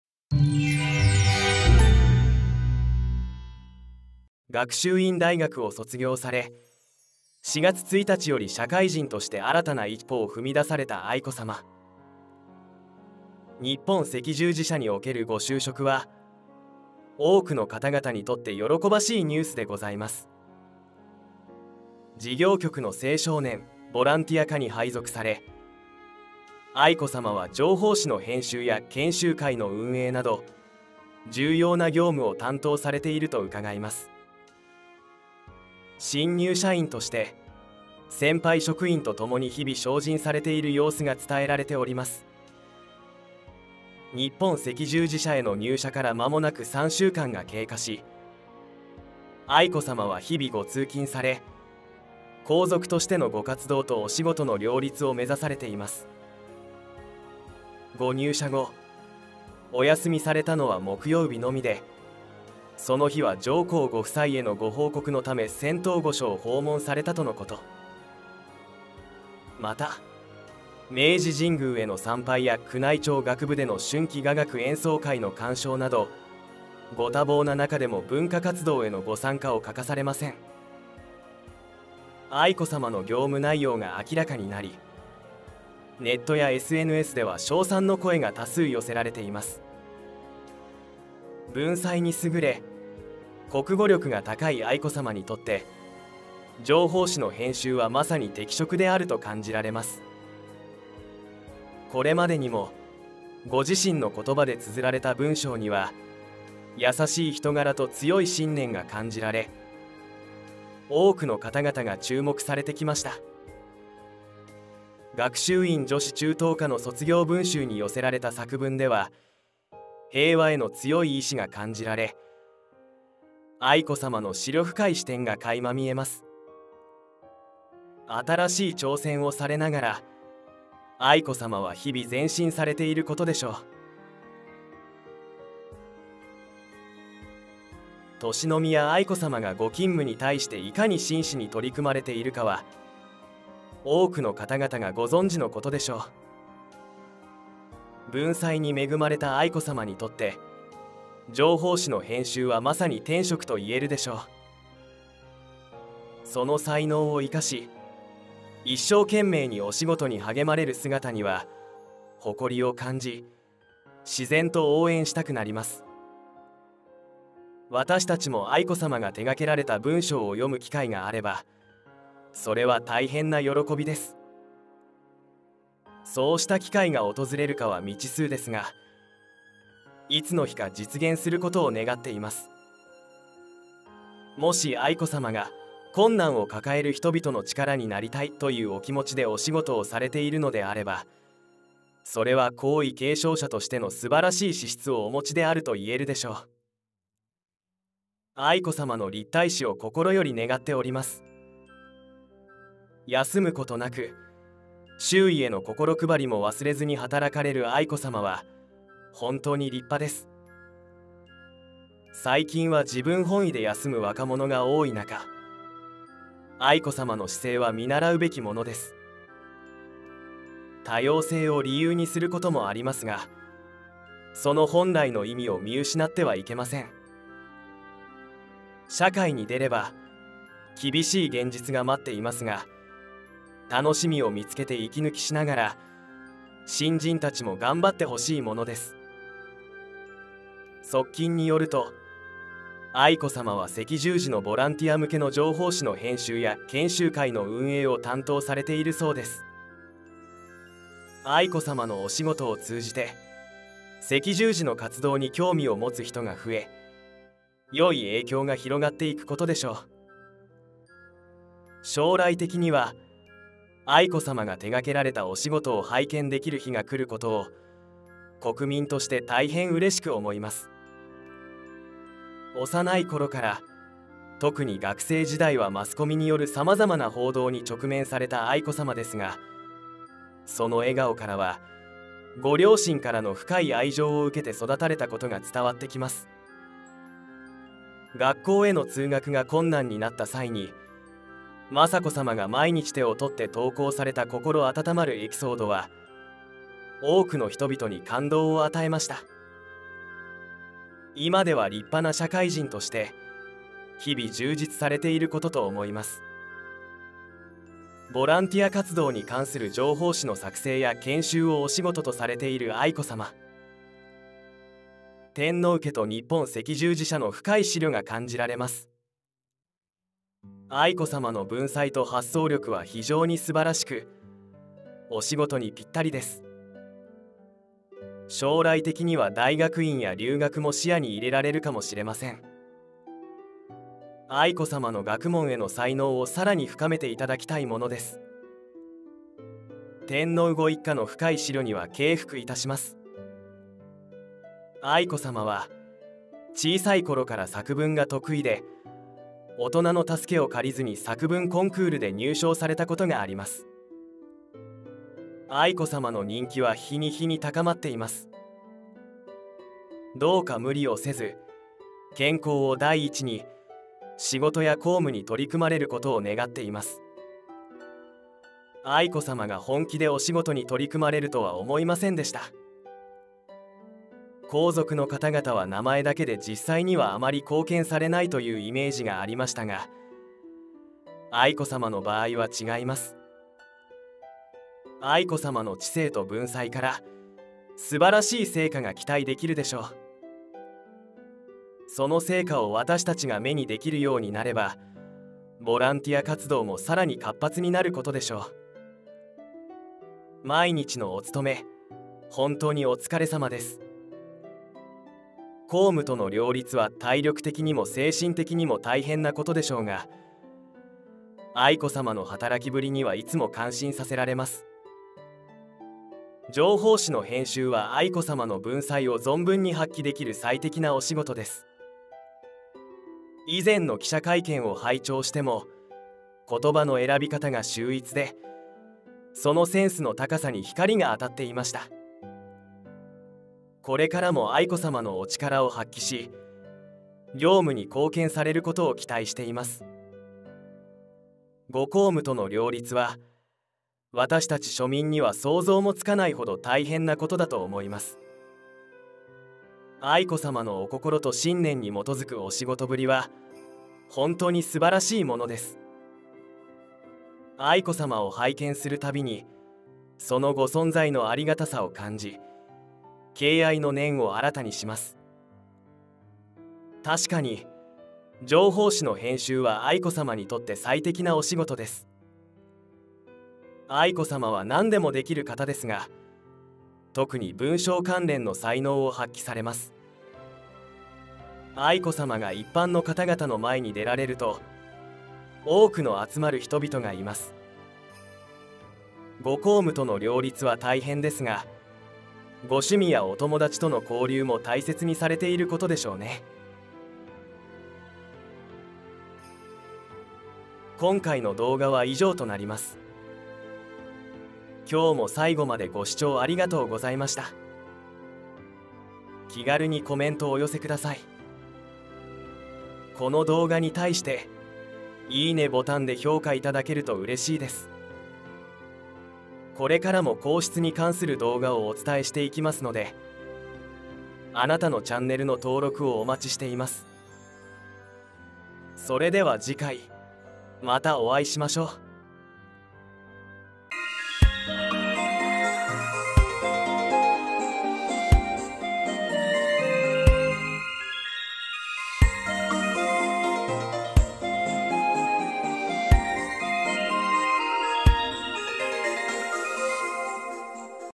愛子さまのお仕事!読んでみたい！ファンにとって待望の内容が明らかに！その反響とは？「愛子さまのお仕事にこれからも注目です！」【皇室Labo】
雅子さま還暦『内なる戦いの30年』

「テムズとともに英国の二年間」

天皇陛下
愛子さま
雅子さま